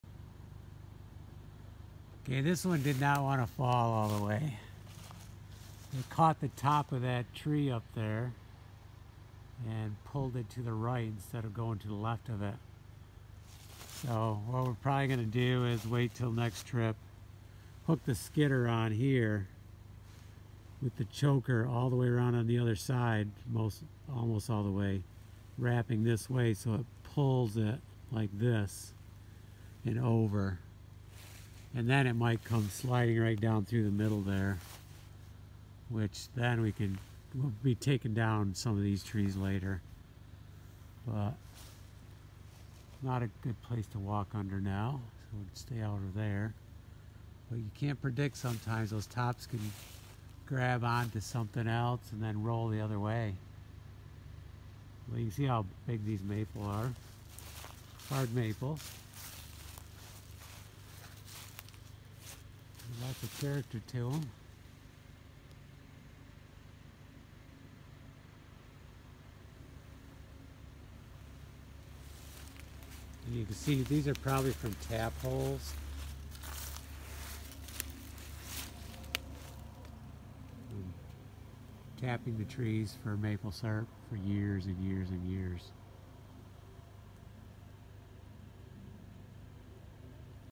Old Large Maples Cut Down Part 2 ... Forest Projects, Lowville NY
From playlist "Miscellaneous Projects Elsewhere"@victorianestate